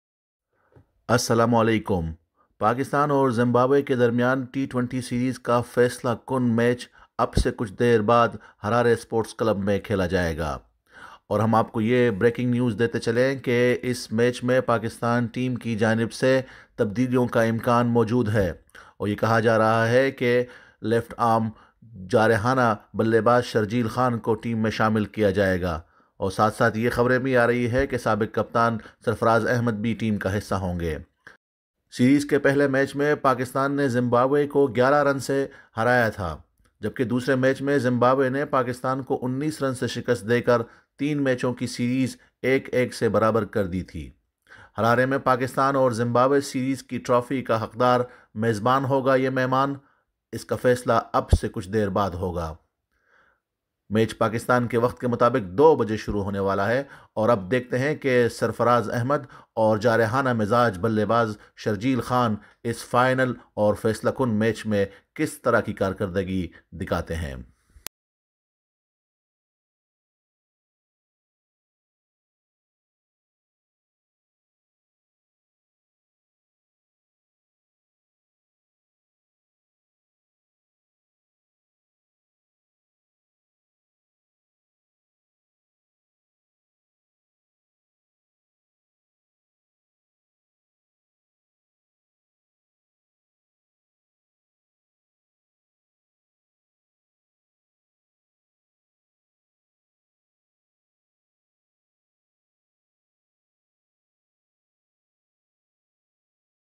Sharjeel Khan and Sarfaraz Ahmed will Play Today Match | PakvsZim 3rd T20
Sarfaraz Ahmed, Hassan Ali and Sharjeel Khan are in the side, out go Danish Aziz, Asif Ali and Arshad Iqbal

Pakistan: 1 Mohammad Rizwan (wk) 2 Sharjeel Khan 3 Babar Azam (capt) 4 Sarfaraz Ahmed 5 Mohammad Hafeez 6 Sharjeel Khan 7 Faheem Ashraf 8 Hasan Ali 9 Mohammad Hasnain10 Haris Rauf 11 Usman Qadir

Zimbabwe: 1 Wesley Madhevere 2 Tarisai Musakanda 3 Sean Williams 4 Tadiwanashe Marumani 5 Brendan Taylor (capt&wk) 6 Ryan Burl 7 Regis CHakabva 8 Luke Jongwe 9 Wellington Masakadza 10 Blessing Muzarabani 11 Rizhard Ngarava